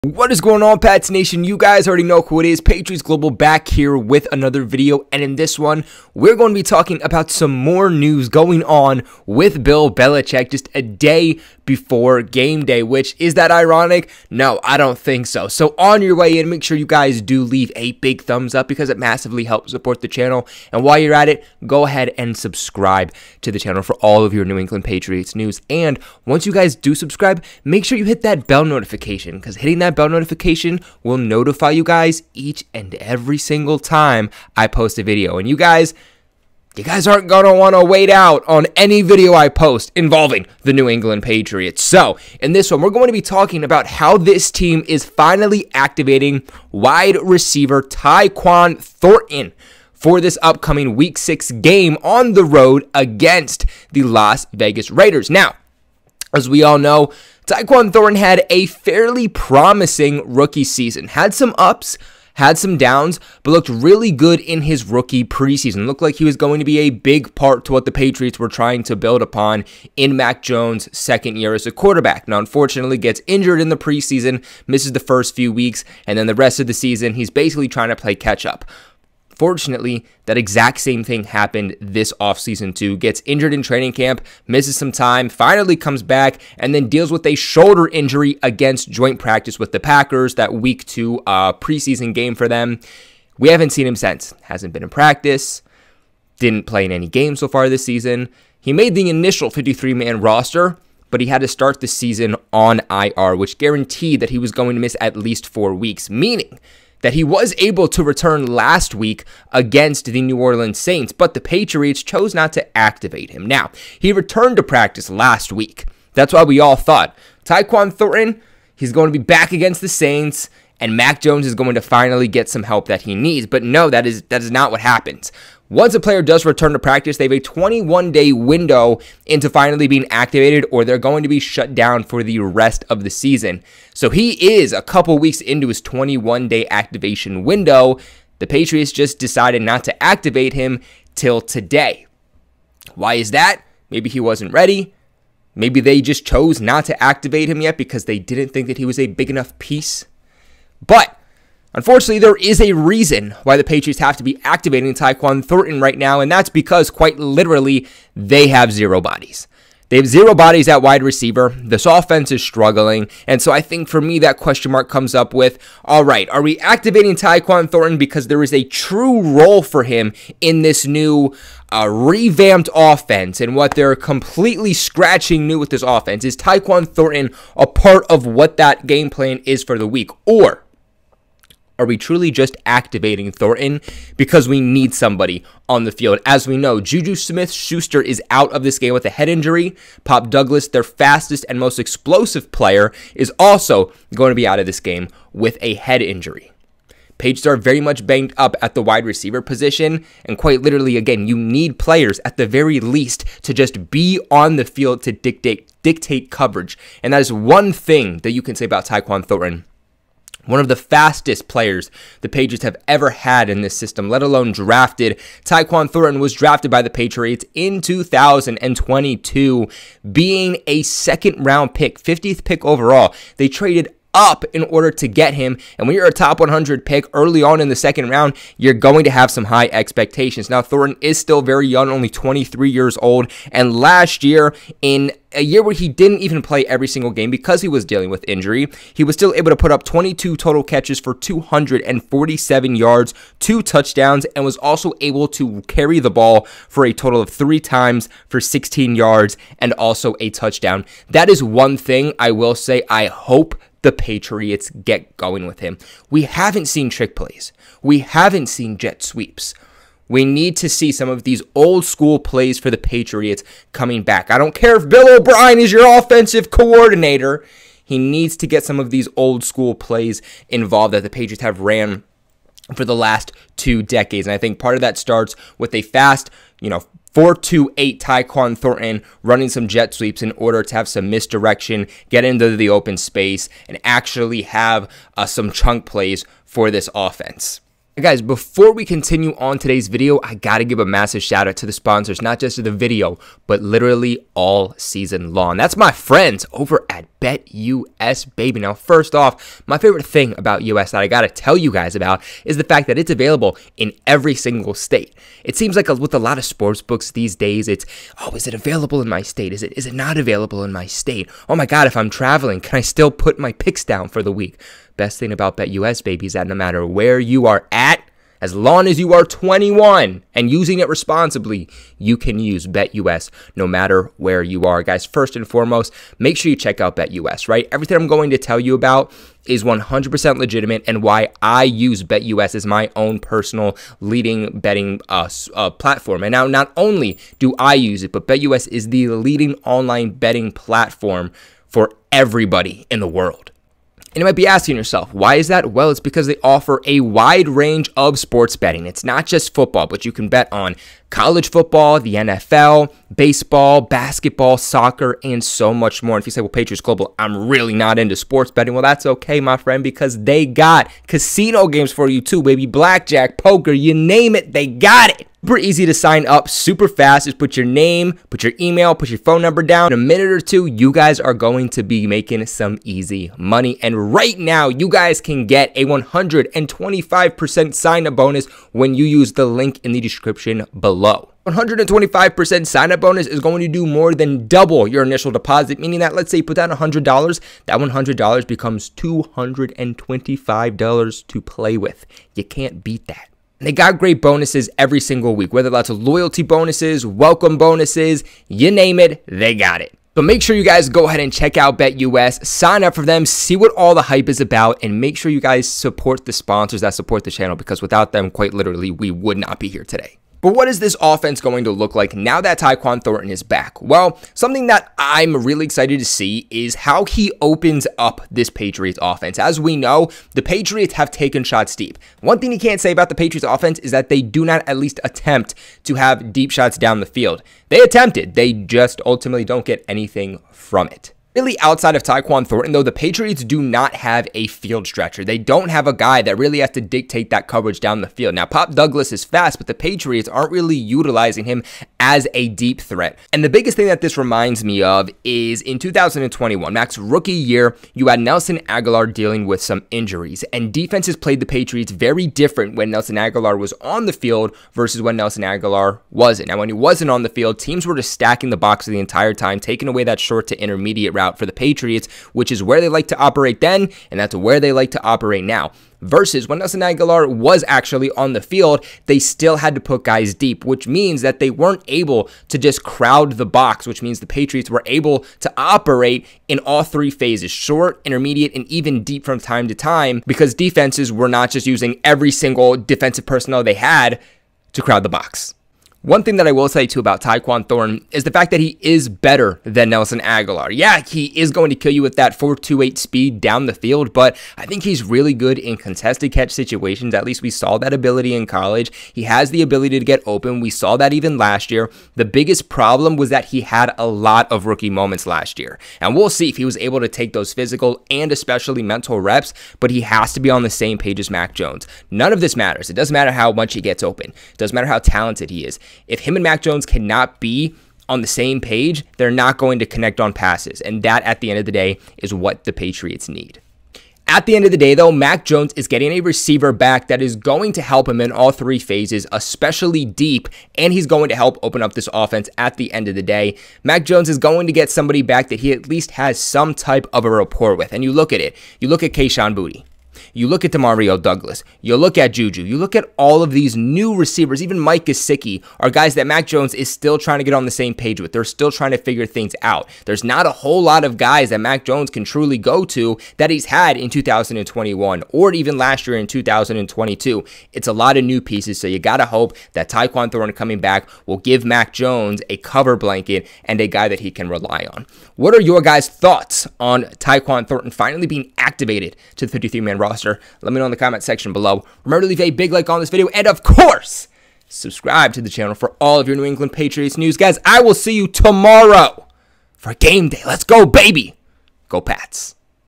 Patriots WR Tyquan Thornton Expected to Play Vs Raiders after being Activated
In todays video I talked about New England Patriots activating wide receiver Tyquan Thornton off of injured reserve and how he’s expected to play against the Raiders. What do you think? In this video we discussed the latest update on the New England Patriots moves.

Tip Jar 💵 (Not mandatory but extremely appreciated as this is my full time job)